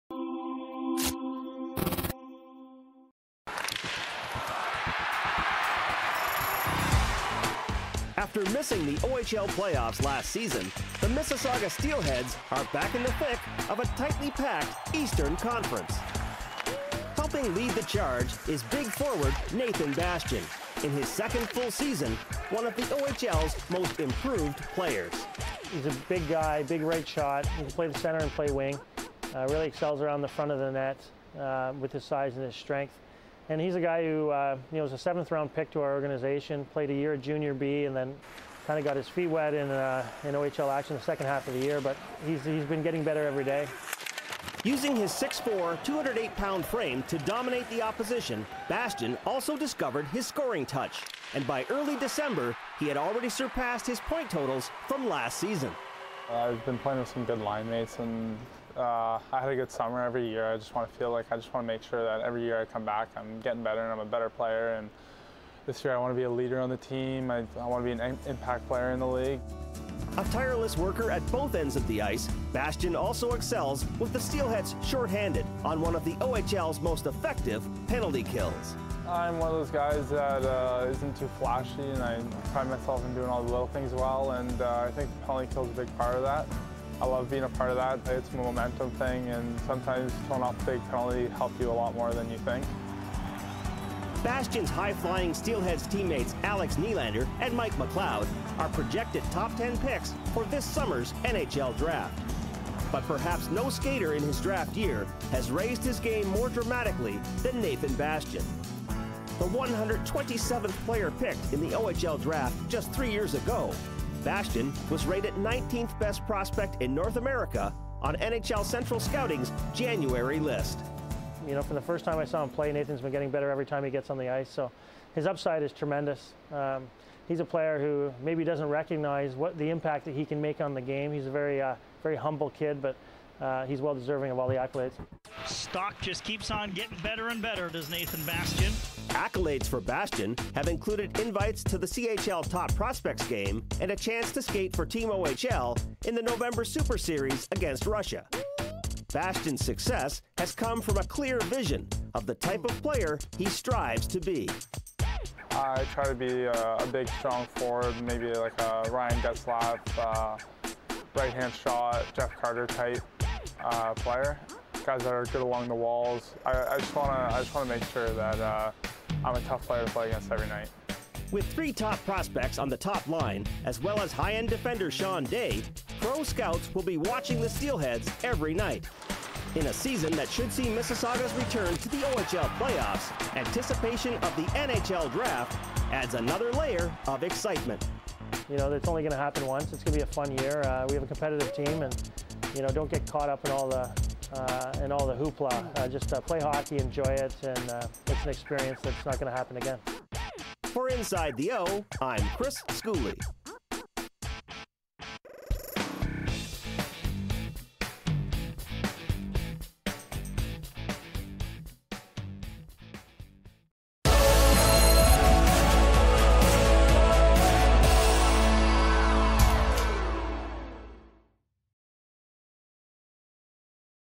Mississauga Steelheads Nathan Bastian: NHL Draft prospect
The 127th player selected in the OHL Draft just 3 years ago, Mississauga forward Nathan Bastian is now a projected First Round pick for the 2016 NHL Draft.